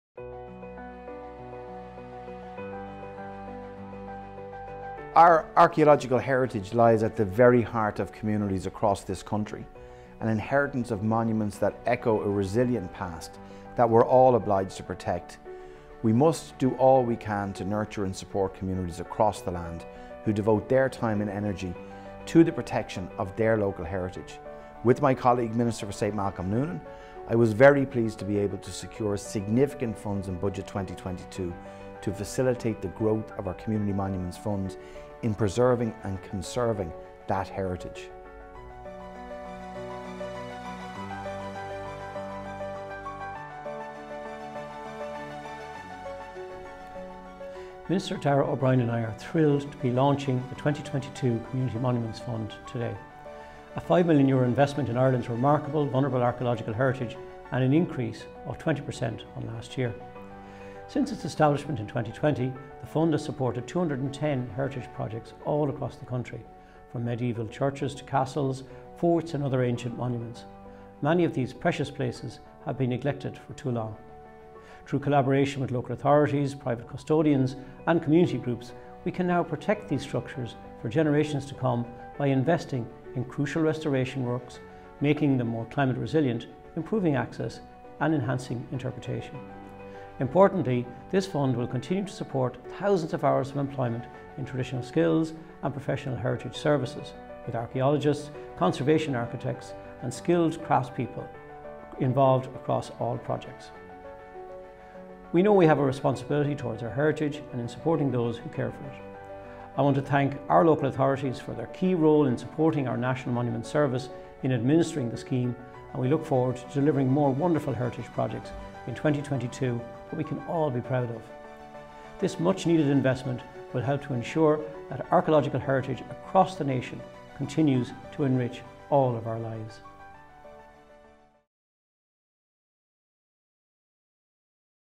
The 2022 Community Monuments Fund is Now Open!
The 2022 Community Monuments Fund is now open for applications!

Started in 2020, the Fund has been a huge success so far – helping us to protect our monuments for the benefit of local communities and visitors.

€5 Million is available for 2022 – money that will not only safeguard our archaeological treasures, but will also support heritage professions and provides job opportunities in archaeology, building conservation and traditional skills.